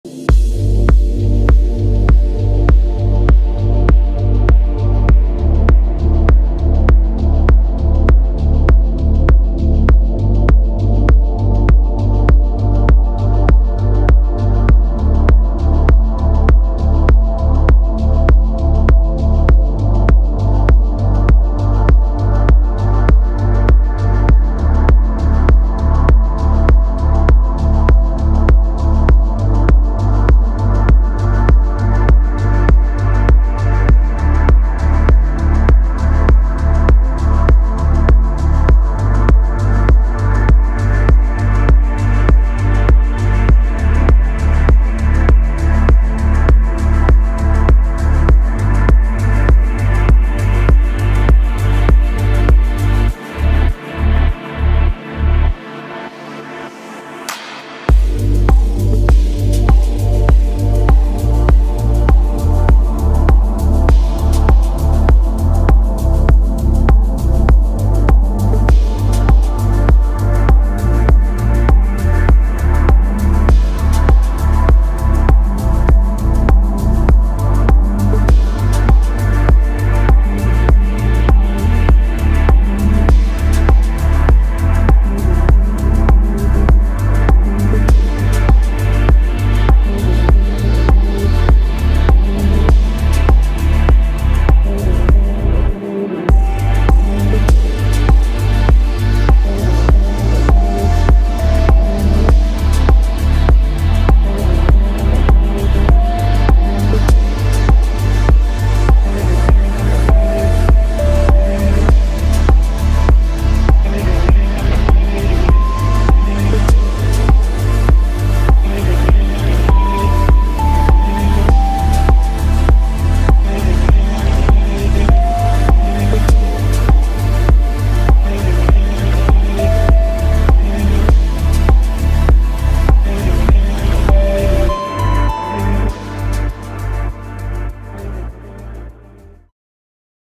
Square "Macedonia" at night | Skopje | North Macedonia 🇲🇰
In this video, viewers can witness the stunning beauty of the "Macedonia" Square in Skopje, North Macedonia, during the nighttime. The footage showcases the grandeur of the square, including its beautiful sculptures. The video takes the viewer on a peaceful tour of the square, allowing them to enjoy the serene atmosphere and the breathtaking views of the surrounding buildings and structures. This video is perfect for those who love to explore urban areas at night and appreciate the beauty of grand architecture and illuminated cityscapes.